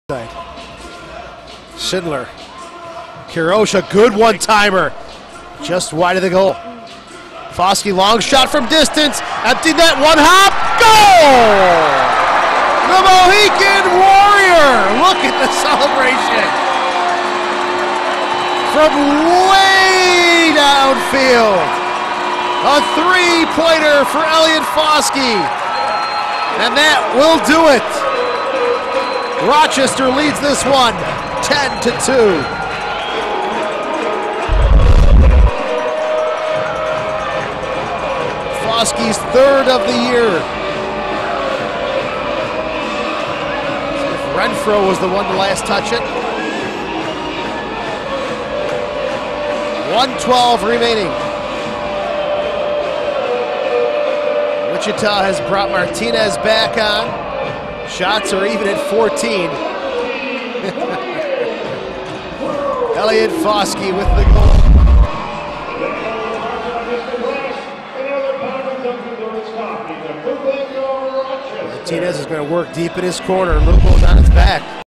2012-02-26 (4) Elliot Fauske
MISL Wichita Wings vs. Rochester Lancers at the Blue Cross Arena at the Rochester War Memorial, Rochester NY